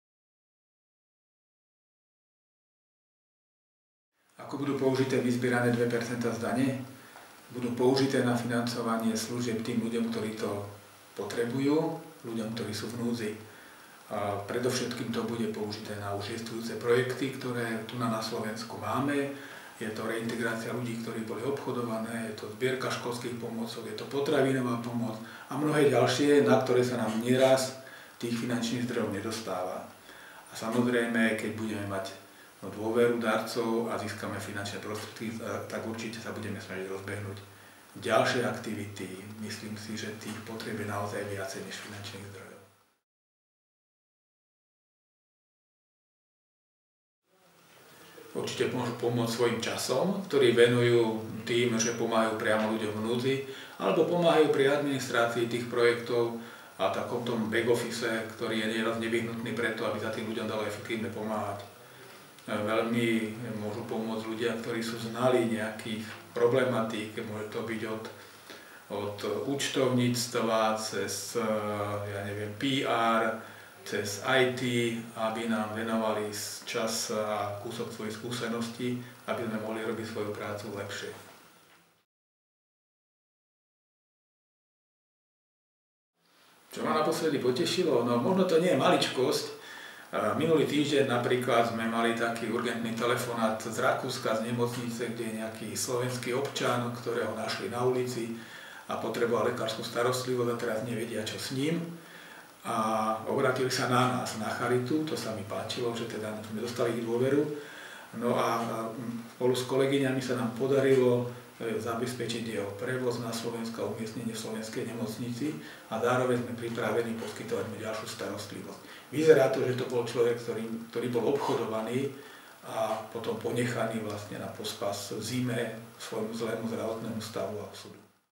Ako využije Slovenská katolícka charita darované 2%?
Slovenská katolícka charita má jasnú víziu ako použije darované 2%.
Inštrukcie k podávaniu daňového priznania z dane z príjmu spolu s potrebnými tlačivami vrátane Vyhlásenia o poukázaní 2% nájdete na našom webe